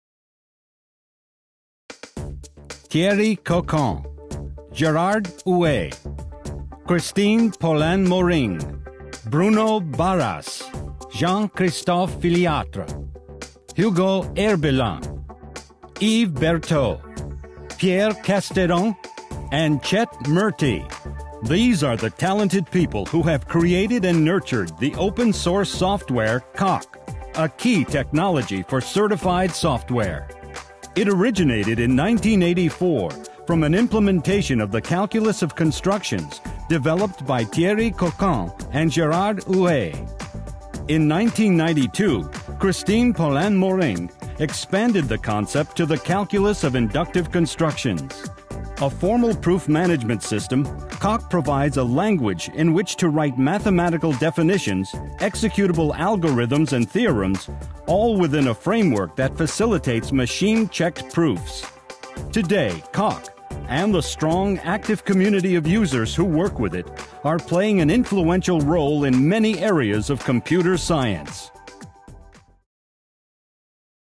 ACM Software System Award 2013 - Coq
Coq Selected As Recipient Of The 2013 Software System Award
Coq is a software tool for the interactive development of formal proofs, which is a key enabling technology for certified software. It provides a formal language to write mathematical definitions, executable algorithms and theorems together with an environment for semi-interactive development of machine-checked proofs. An open source product, Coq has played an influential role in formal methods, programming languages, program verification and formal mathematics.